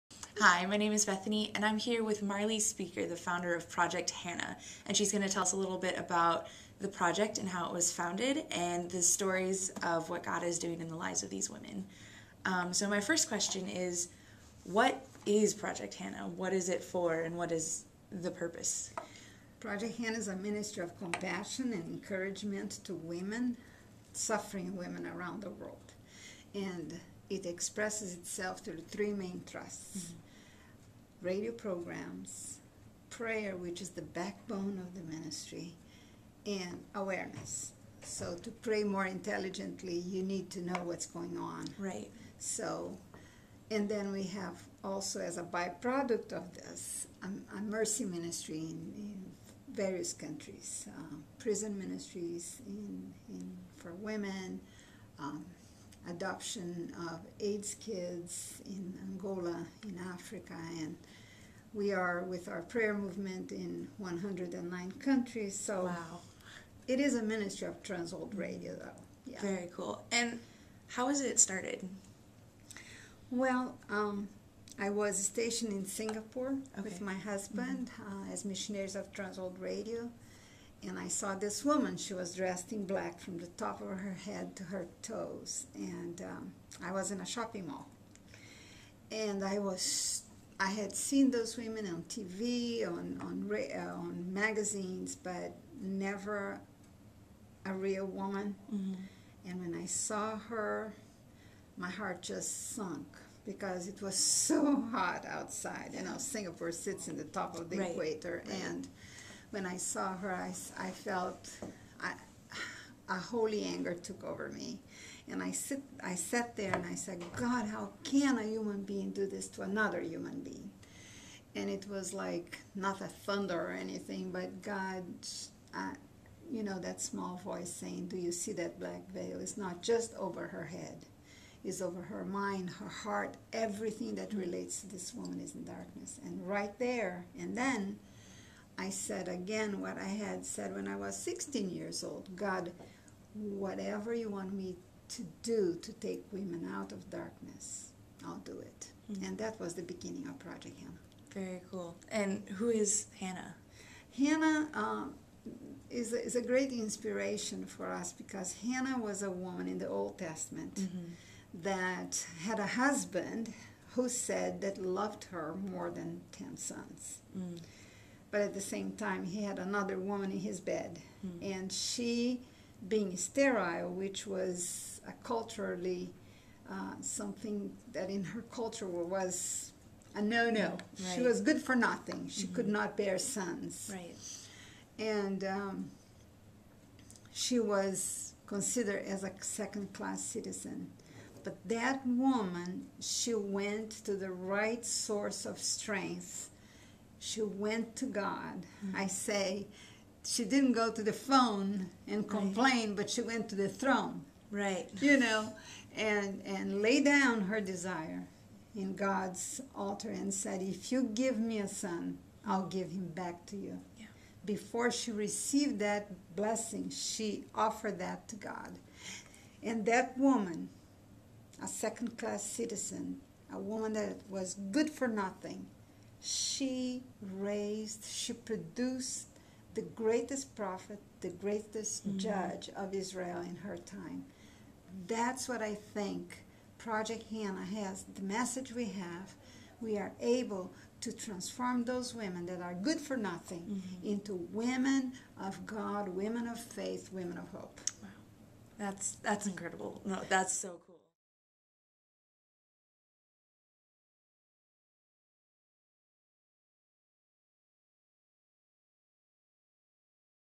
Project Hannah told by Marli Spieker
Interview with Marli Spieker about Project Hannah